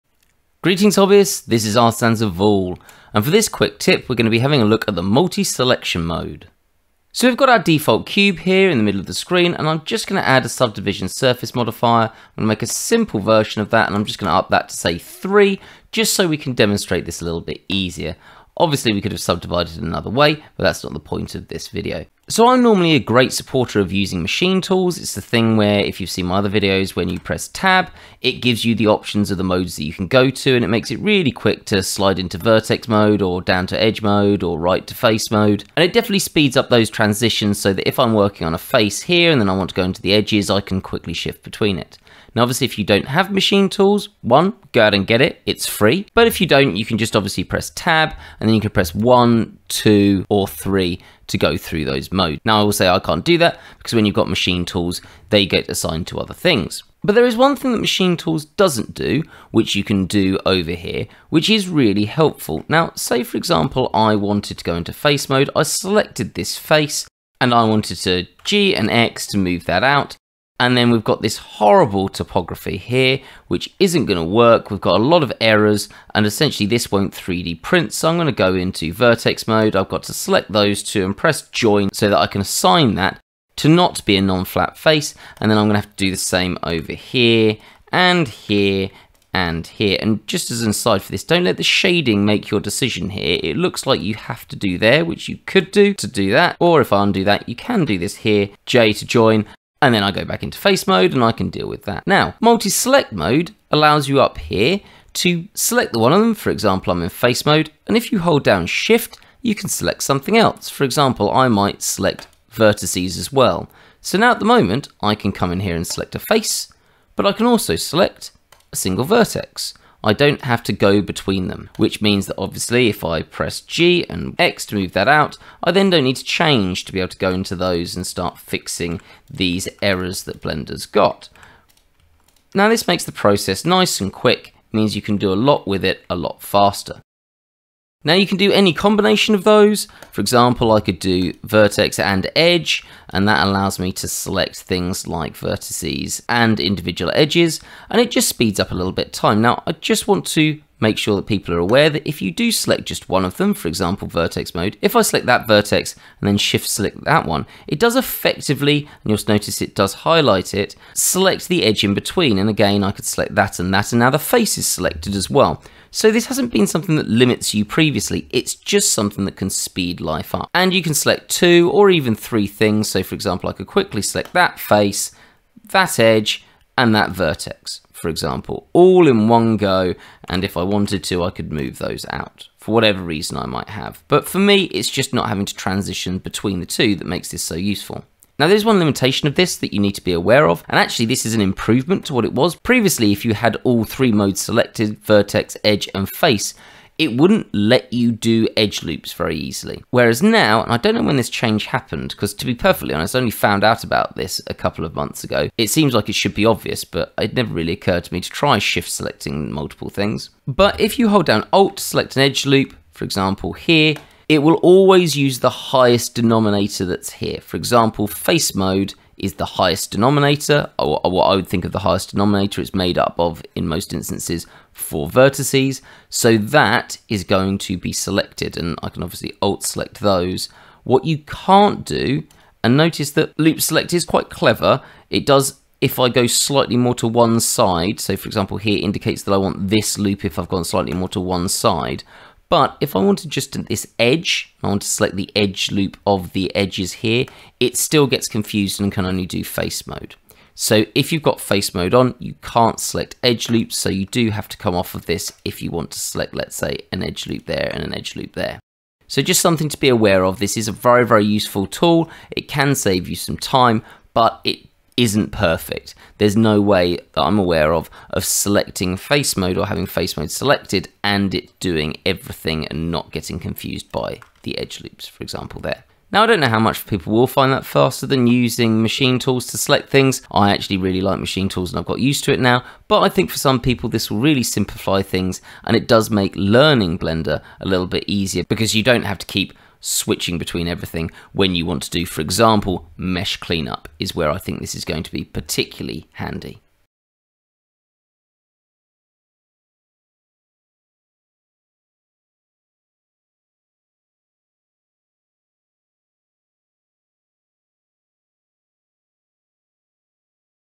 Multi Selection Mode in Blender
A quick look at multi selection mode, where I would be most likely to use it and its limitations.